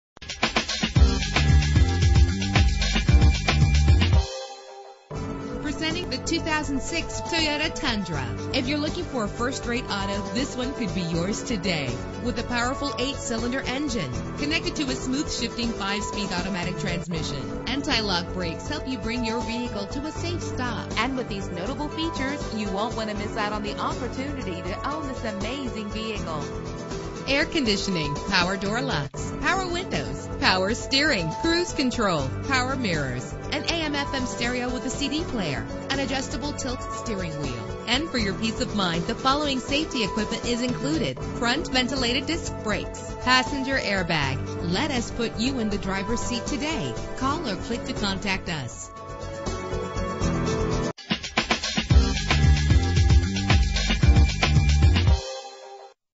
2006 Toyota Tundra - Ruidoso NM
Year: 2006
Make: Toyota
Model: Tundra
Trim: SR5
Engine: 4.7 liter 8 cylinder 32 valve
Transmission: 5-Speed Automatic
Color: Phantom Gray Pearl
Mileage: 100300
Address:
304 Hwy 70